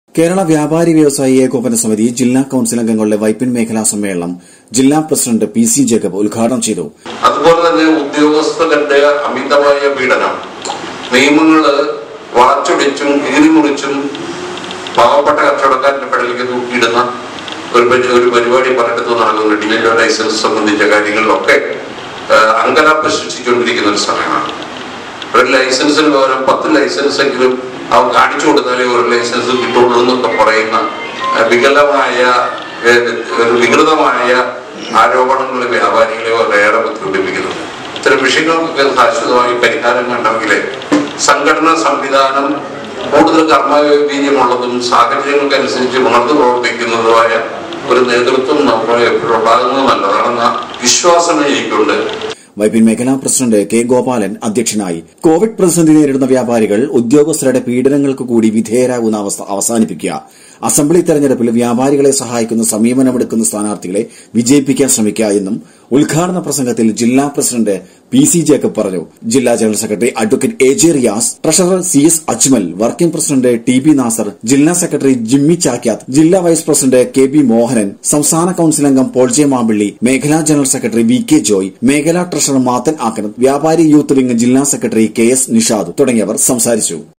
കേരള വ്യാപാരി വ്യവസായി ഏകോപന സമിതി കണ്‍വെന്‍ഷന്‍ നടത്തി.
കേരള വ്യാപാരി വ്യവസായി ഏകോപന സമിതി ജില്ലാ കൗണ്‍സില്‍ അംഗങ്ങളുടെ വൈപ്പിന്‍ മേഖല സമ്മേളനം ജില്ലാ പ്രസിഡന്റ് പി. സി. ജേക്കബ് ഉത്ഘാടനം ചെയ്തു.